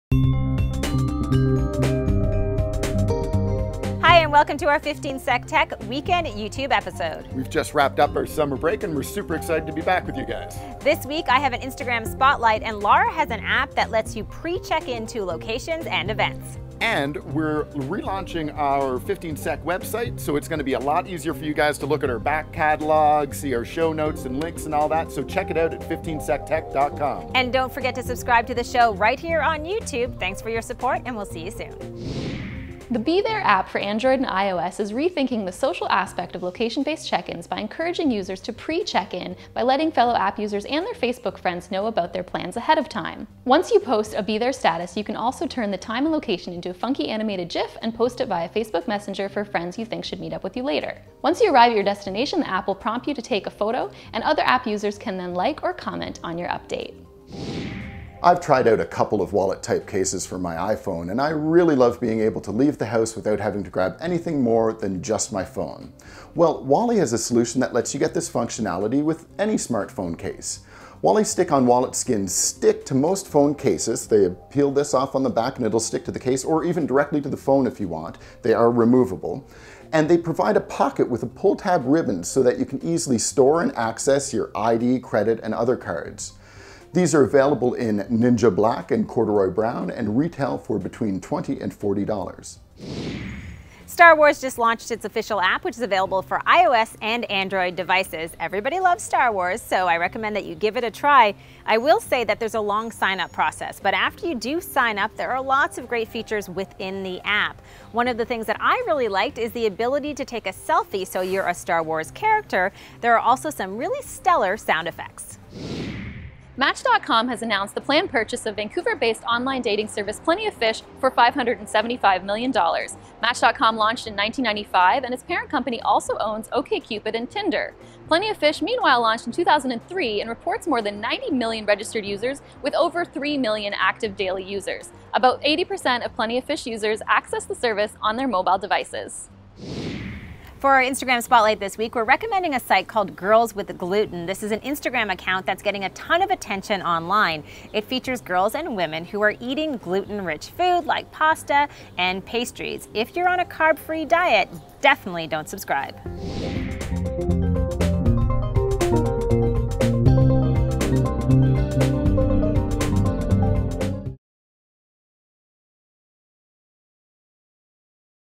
15secTech wk1507a: Smartphone wallets, Star Wars & BeThere apps, online dating, and more!
The team is back from summer break! Amber kicks things off with the official Star Wars app for Android and iOS, plus a spotlight on the Instagram account @GirlsWithGluten. Lara brings us the news that the Match group (of Match.com, OkCupid, Tinder, etc.) plans to buy Canadian dating service PlentyOfFish, and talks about her recent use of the pre-check-in app, BeThere. Plus Jeff tries out a Wally stick-on wallet skin from Distil Union.

What tech or gear is making your summer great?